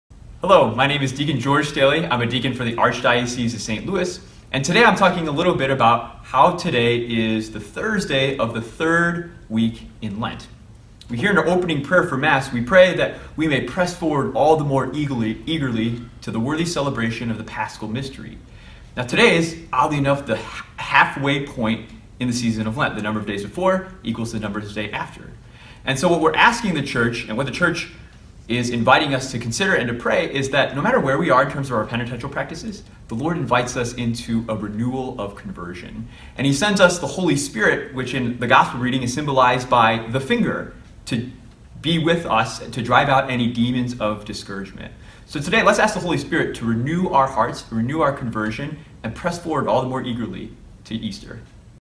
Press Forward All the More Eagerly
Today is the halfway point of Lent. Deacon George Staley, Theology IV - St. Louis, encourages us to, "ask the Holy Spirit to renew our hearts, renew our conversion and press forward all the more eagerly to Easter."

*This video was filmed in the Homiletics Classroom - a classroom containing a small altar, casket, confessional, and other items pertaining to ordained ministry. This classroom is used by seminarians preparing for the transitional diaconate and sacred priesthood.

This is a part of a weekly reflection series presented by seminarians during the season of Lent.

Kenrick-Glennon Seminary
5200 Glennon Dr.